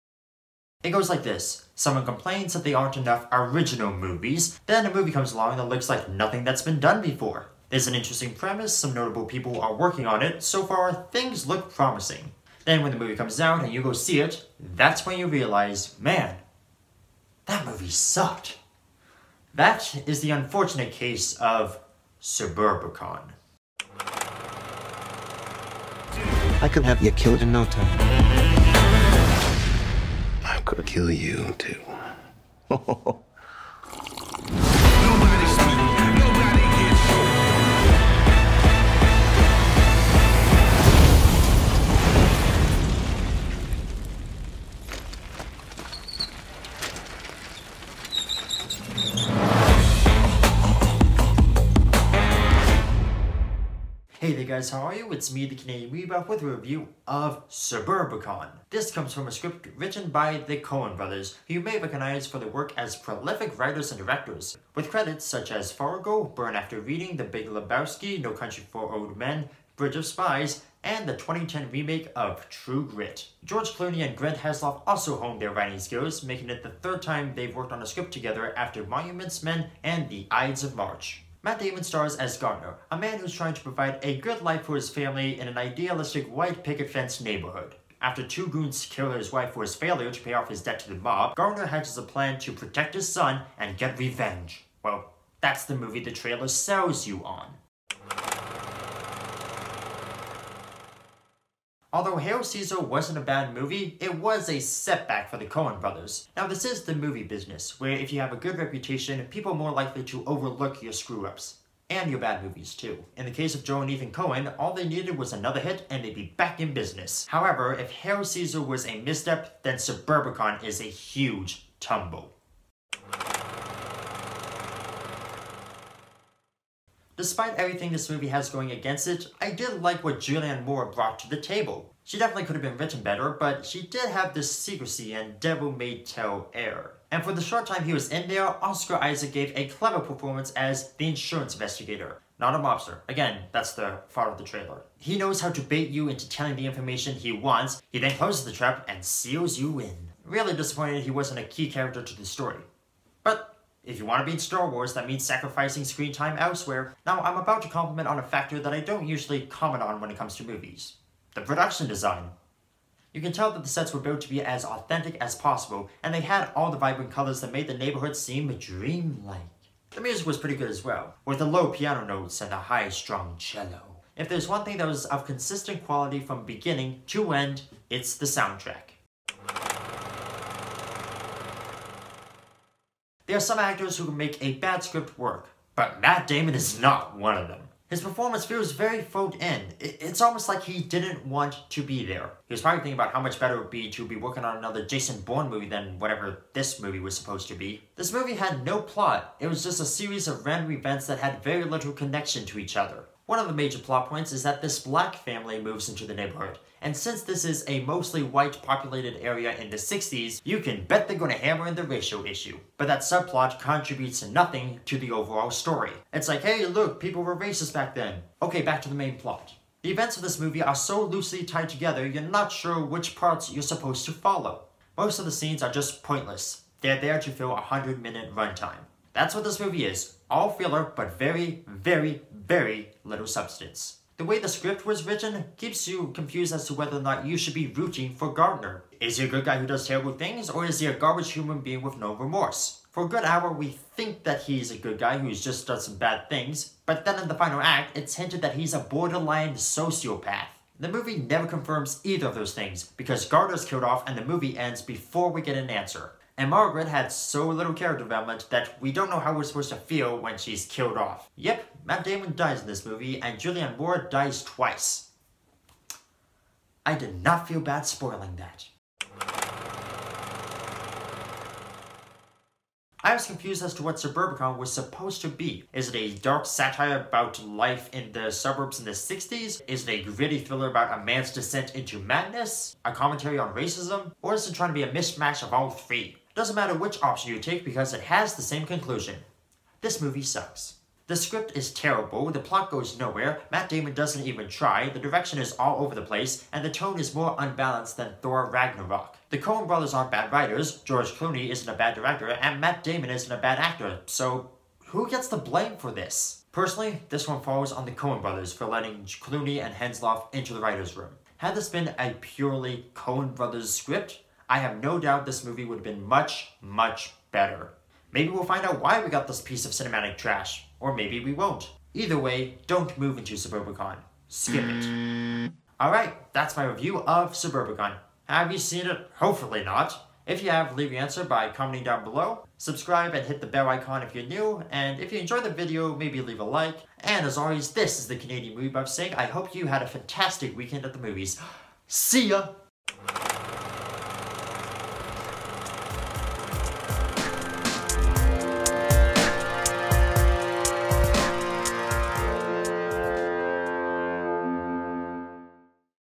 Suburbicon - Canadian Movie Buff Reviews! Epi.63
The Coen brothers try to shake off the under-whelming "Hail Caesar!" with this dark comedy, but it ultimately ends up being one of their worst movies yet.

My Twitter! Where I blurb out nonsense.